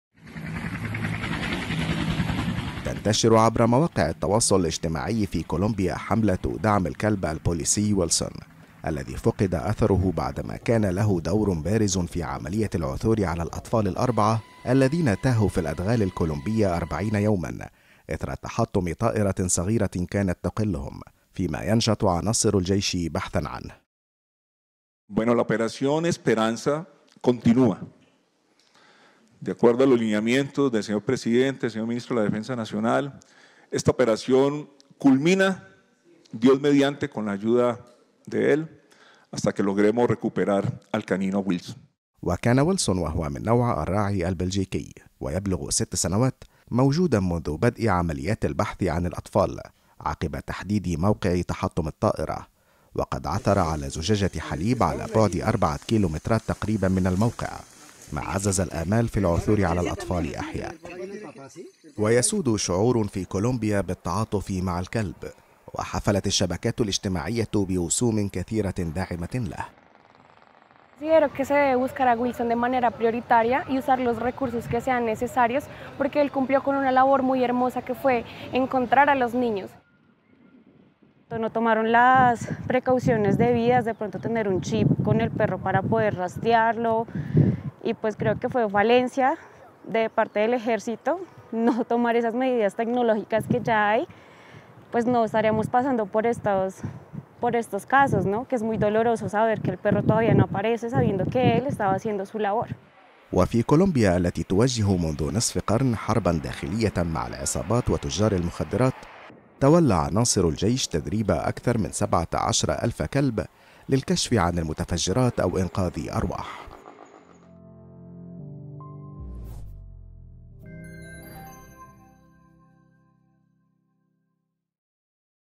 كولومبيا تطلق عمليات بحث حثيثة عن كلب ساهم بالعثور على 4 أطفال
أطلق العديد من مستخدمي مواقع التواصل الاجتماعي، حملة دعم لإنقاذ الكلب البوليسي "ويلسون"، الذي فُقد أثره بعدما كان له دور بارز في عملية العثور على الأطفال الأربعة الذين تاهوا في الأدغال الكولومبية 40 يوماً، فيما يكثف الجيش عمليات البحث للعثور عليه.